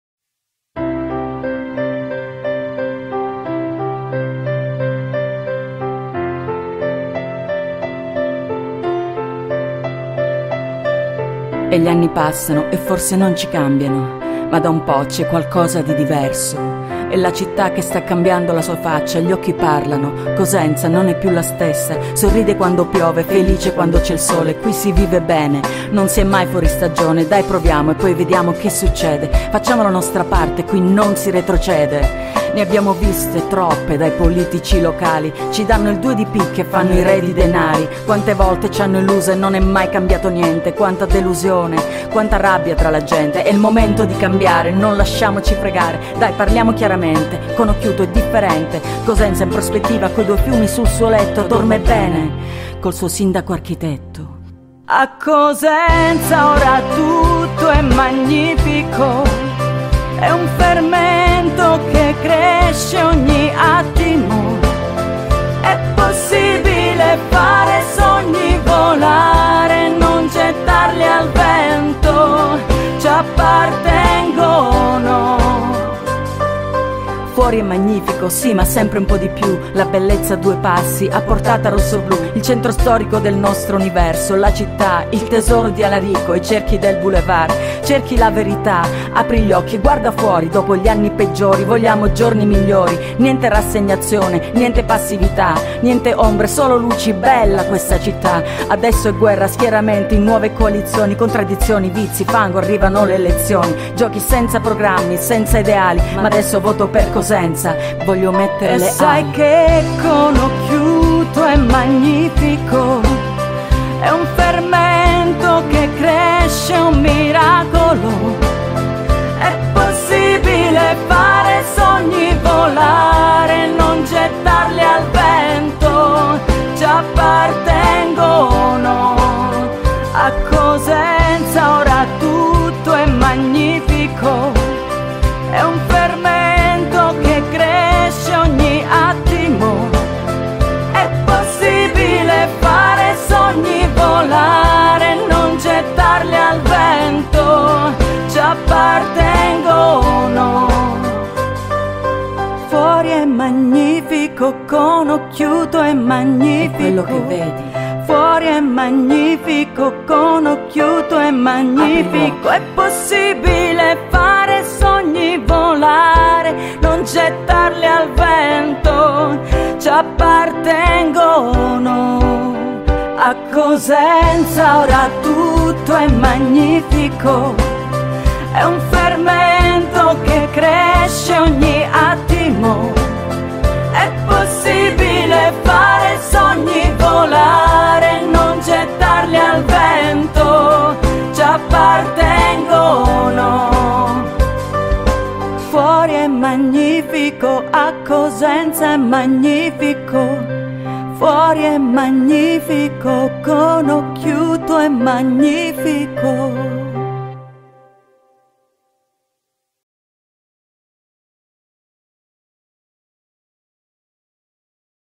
OMAGGIO DI ROSA MARTIRANO
Un emozionante ed inaspettato brano composto da Rosa Martirano, dedicato a me e alla città.
Ascoltatelo, è bellissimo.
Grazie di cuore, Rosa.